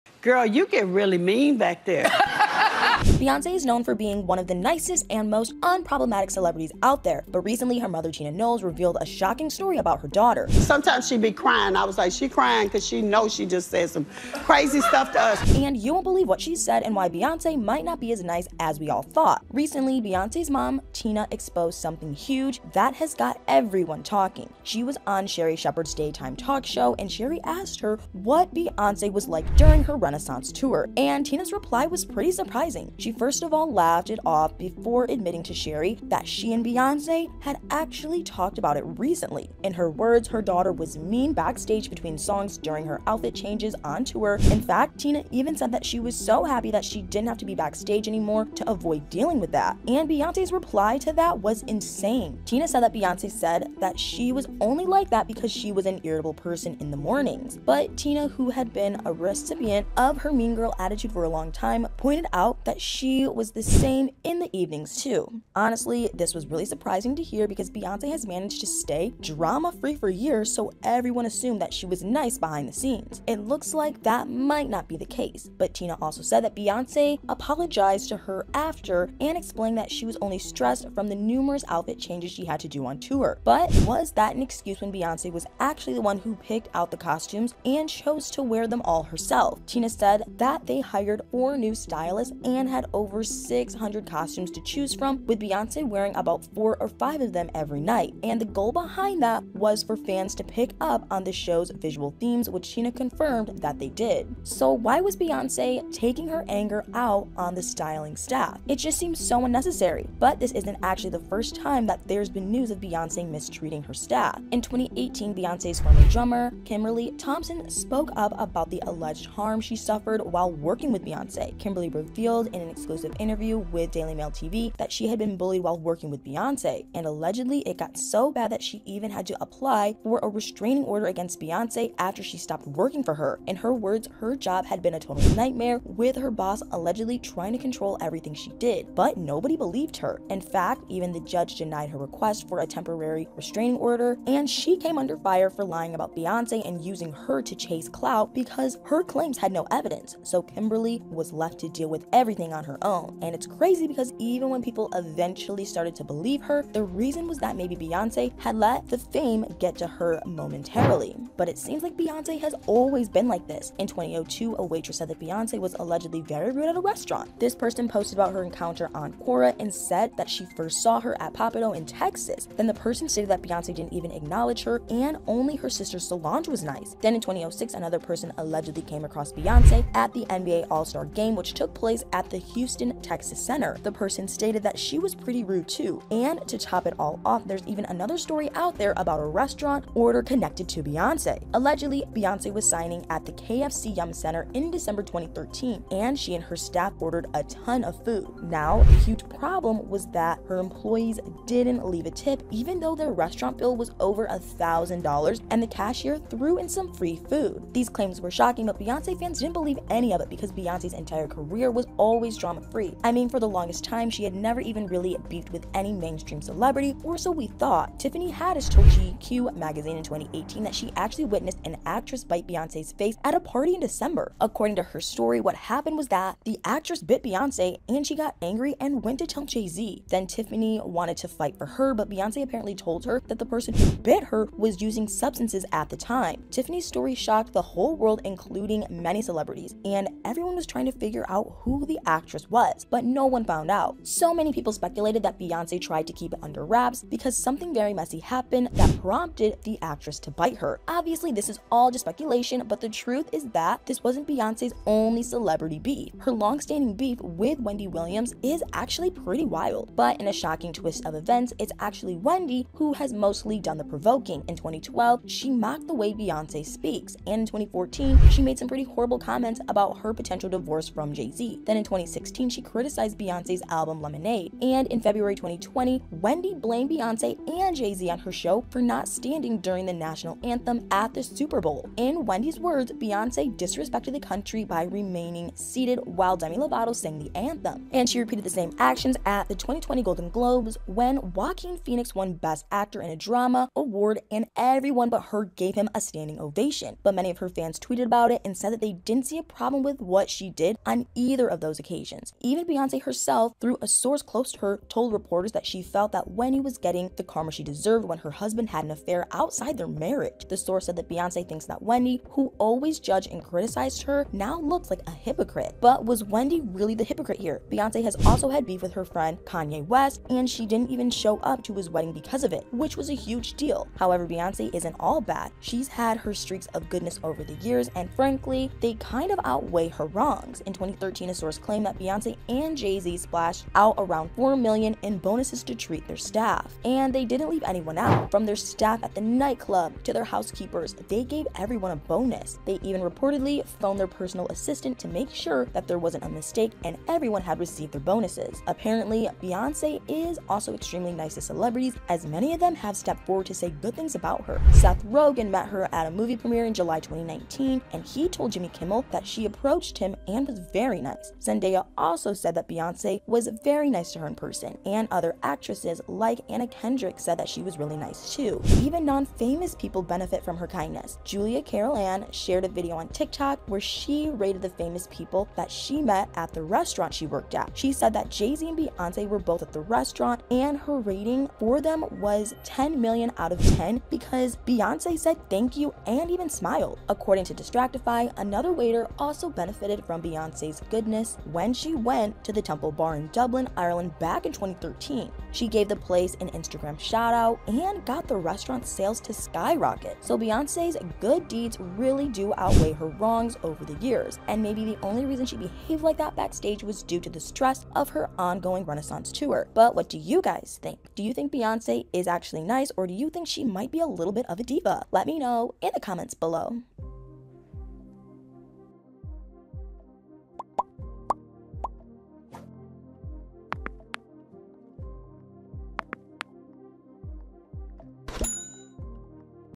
Tina Knowles Exposes Beyonce
Tina Knowles Unveils the Untold Side of Beyoncé

This video is about how Tina Knowles has exposed Beyonce's mean behavior. She has witnessed some pretty bad behavior from Beyonce behind the scenes.